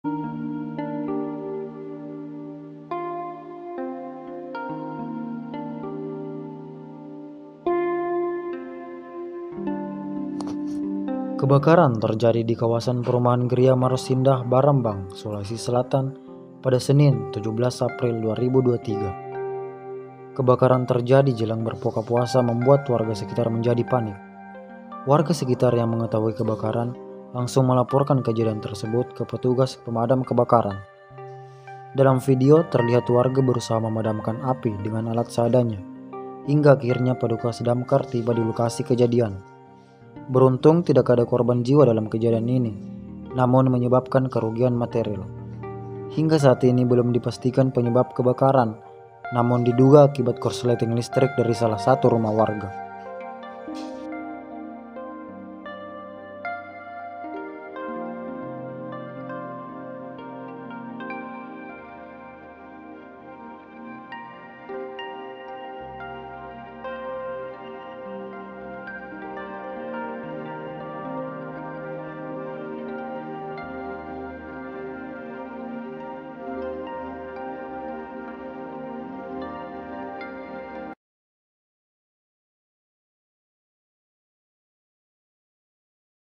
Diduga Akibat Korsleting Listrik, Sejumlah Rumah Warga Ludes Dilalap Api di Maros
Kebakaran terjadi di Kawasan Perumahan Griya Maros Indah (Barambang), Sulawesi Selatan, Senin (17/4/2023).

Kebakaran terjadi jelang berbuka puasa  membuat warga sekitar menjadi panik.

Warga sekitar yang mengetahui kebakaran langsung melaporkan kejadian tersebut ke petugas pemadam kebakaran.

Dalam video terlihat warga berusaha memadamkan api dengan alat seadanya, hingga akhirnya petugas damkar tiba di lokasi kejadian.

Beruntung tidak ada korban jiwa dalam kebakaran ini, namun menyebabkan kerugian materil.

Hingga saat ini belum dipastikan penyebab kebakaran, namun diduga akibat korsleting listrik dari salah satu rumah warga.

-Muhammad Rafli (Peserta Magang Journalist)
Video terkini dan terbaru..